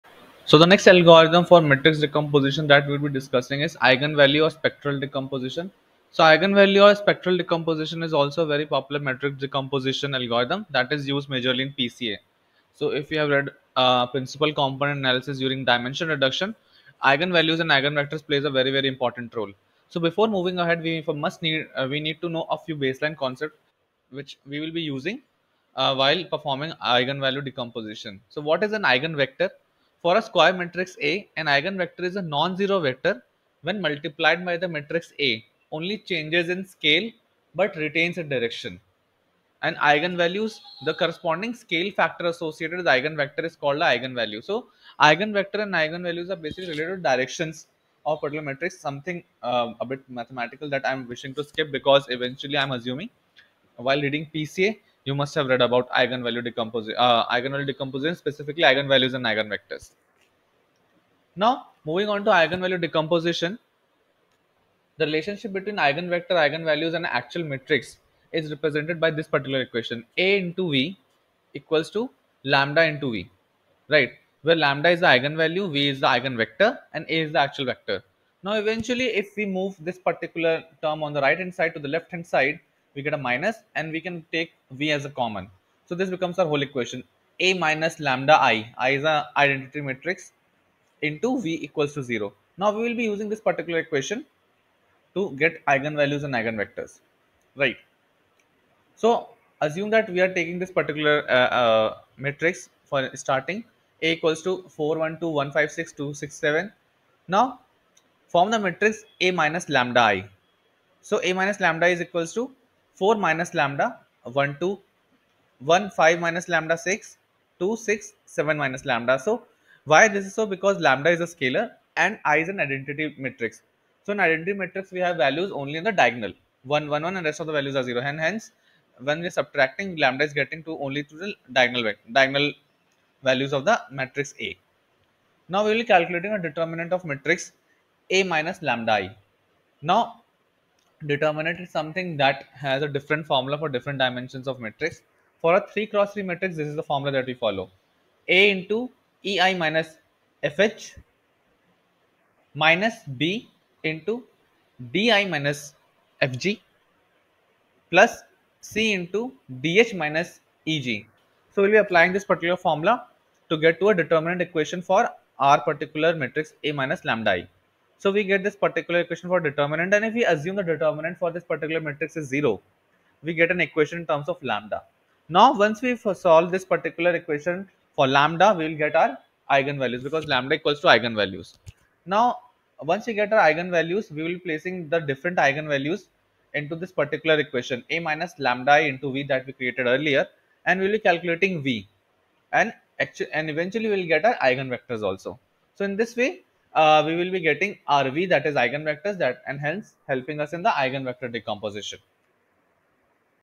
Eigenvalue decomposition explained with example
Eigenvalue decomposition is a method for Matrix Factorization which uses eigenvalues and eigenvectors for decomposing a given Matrix.